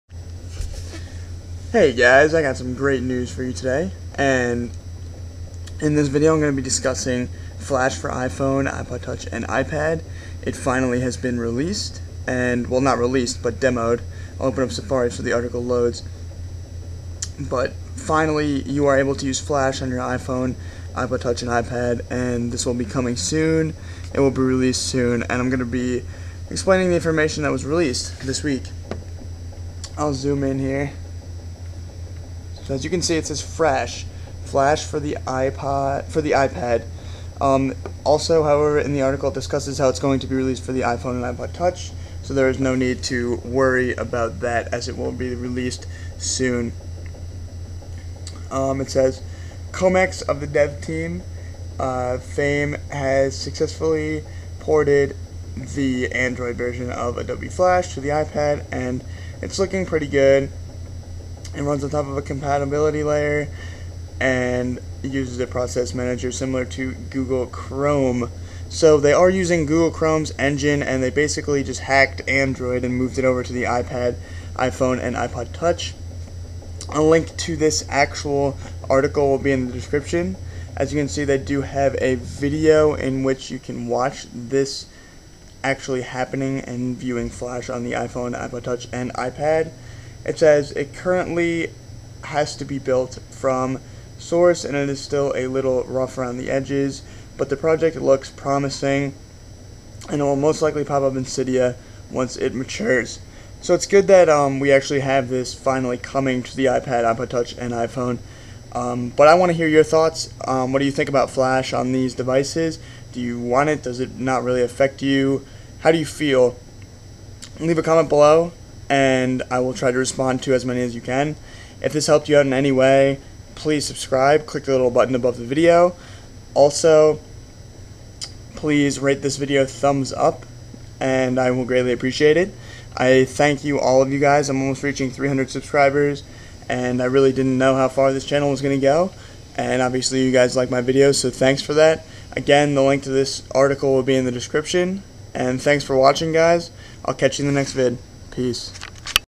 Flash For iPad, iPhone, iPod Touch on iOS 6.1/6/5/4 (Frash)
Flash is coming for iOS 4 on the iPad, iPhone, and iPod Touch! The project is currently being developed by a Developer who is code-named "Comex" and he is porting the Android Flash package over to the iPhone.